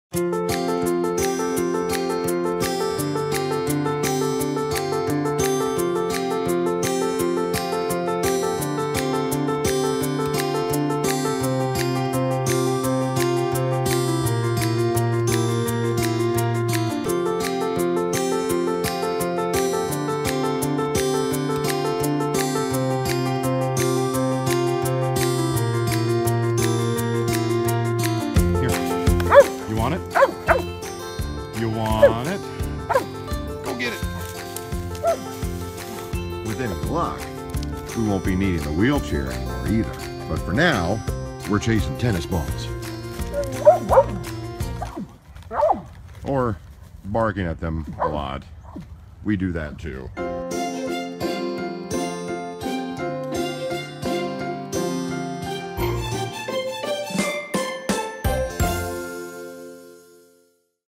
The inspiring tale of Maverick the golden retriever
"Everybody Loves Maverick" is the name of this golden retriever's popular Facebook page, because it's true. Maverick has gone through two separate bouts of cancer and lost the use of his back legs, but his attitude has never been anything but positive. Everyone who meets the Mav cannot help but be inspired by him.